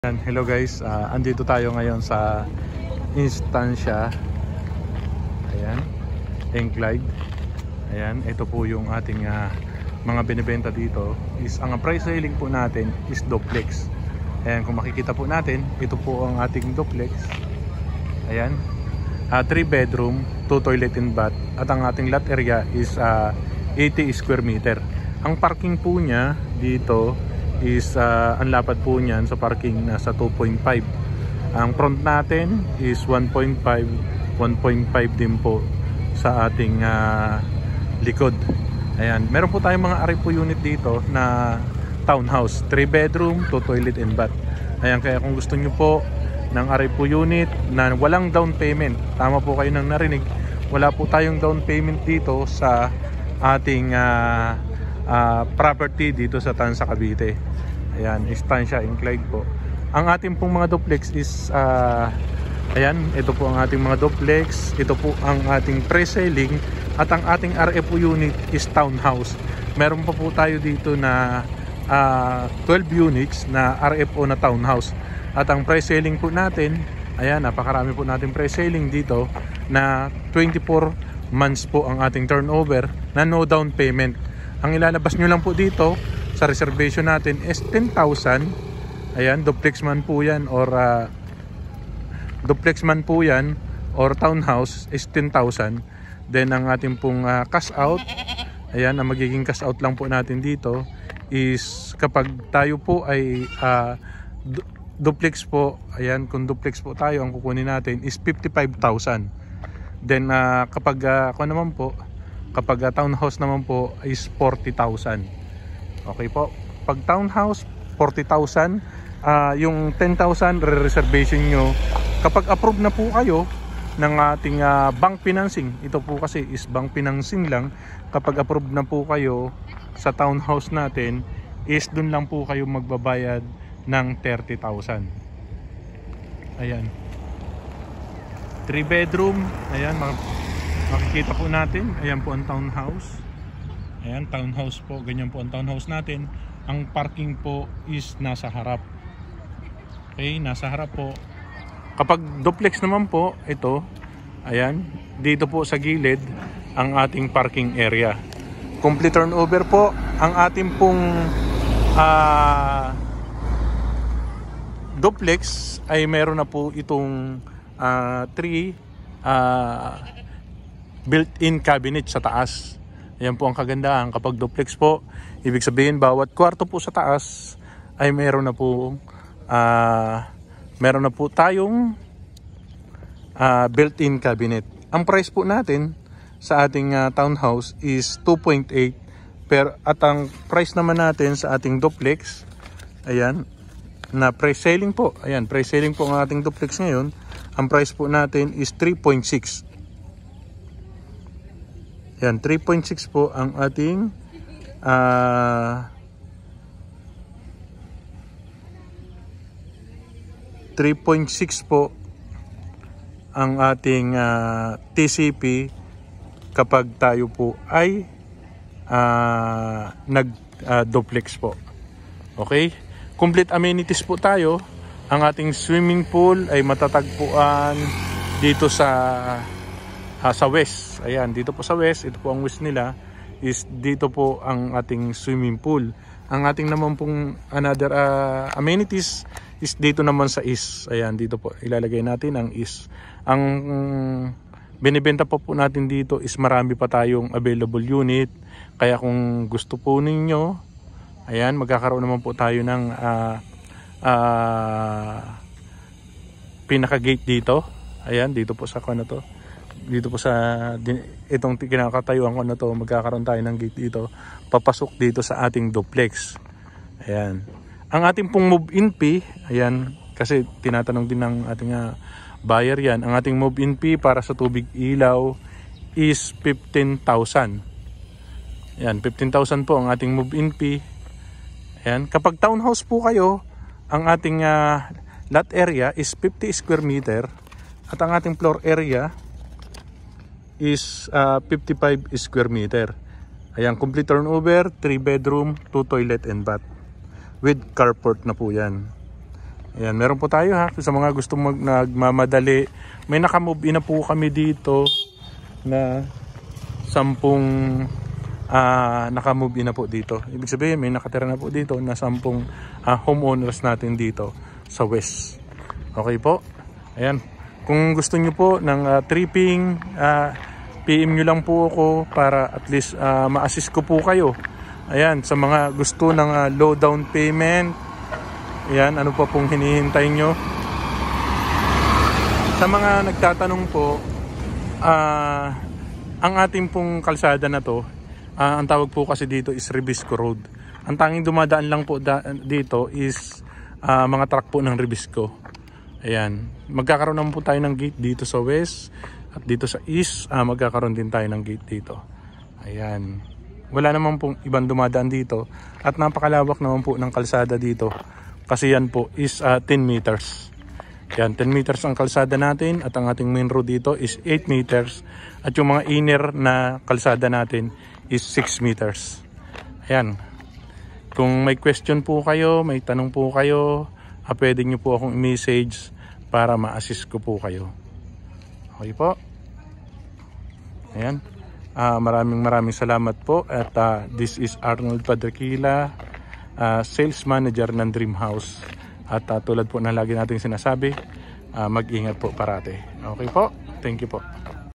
ESTANZIA ENCLACE
RFO Reserve Now At
Estanzia Enclave Tanza
Along The Highway

Accessible Via Cavitex
Thru MOA & Manila Areas

ESTANZIA  ENCLAVE
Brgy. Halayhay, Tanza, Cavite
Ready For Occupancy 10 units left

Complete turn over with cabinet
- Lilipatan mo nlang.

Townhouse Inner - RFO
Duplex - Pre Selling
Quality materials
Along the National Highway
Hindi crowded na community
Modern elegant design
Complete Finish turnover

Inner Unit
Lot Area - 50qm

BANK  FINANCING
Cashout : P40,000

For more information Tripping Reservation